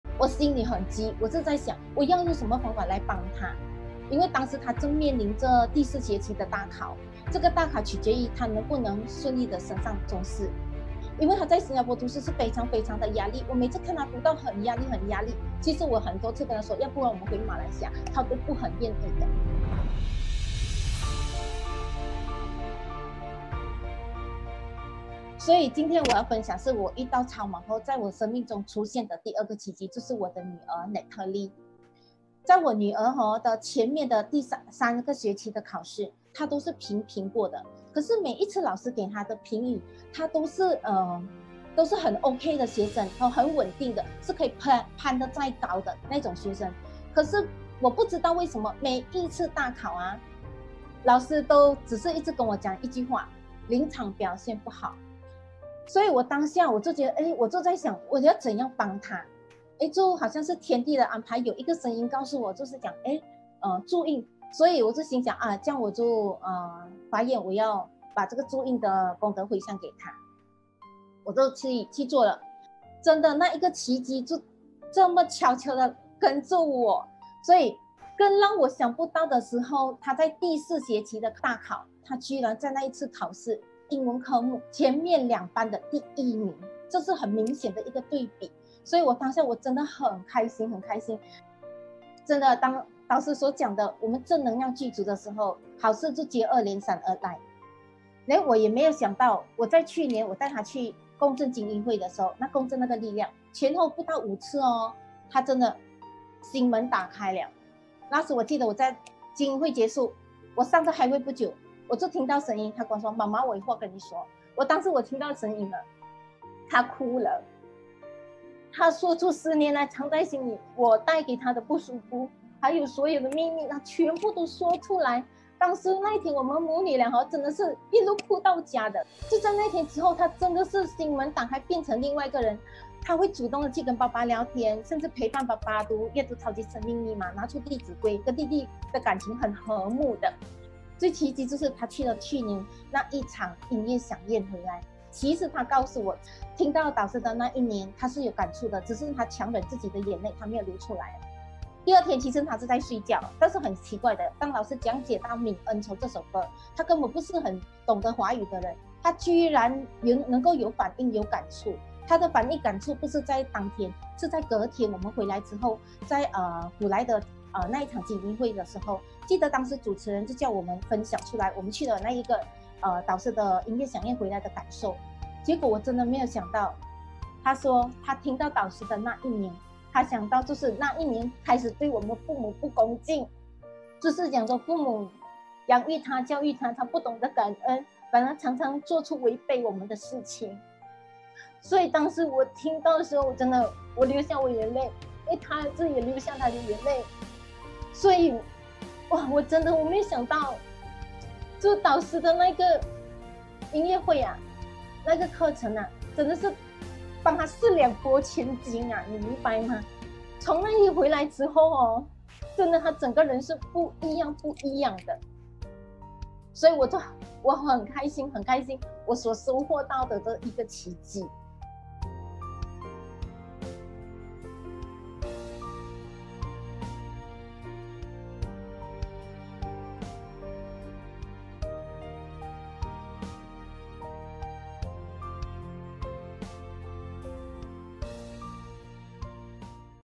【母女關係】那一晚母女倆，解開多年心結 美婷 youtube
母女關係》為人父母，你是不是都會為孩子的成績啊，德行啊，與人相處如何啊而擔心？看著孩子的壓力很大，卻不知道怎樣可以幫助他。感恩超級生命密碼的音樂響宴與課程，讓孩子得到療愈，願意和媽媽說出心結，與家人關系和睦了，更因為心門打開後，成績突飛猛進，讓媽媽感到十分欣慰。

《Mother Daughter Relationships》As parents, aren't we always worry for our children whether they are able to survive in this real world? Seeing them facing lot of pressure,but we don't know how to help them to ease their tension? Gratitude to Super Life Secret Code's musical events and workshops, 美婷, as a mother was able to help her daughter to open up her heart, and healed her body, mind and soul and also to excel in her study.

在愛中共振，在感恩中連結，在祝福中升陽。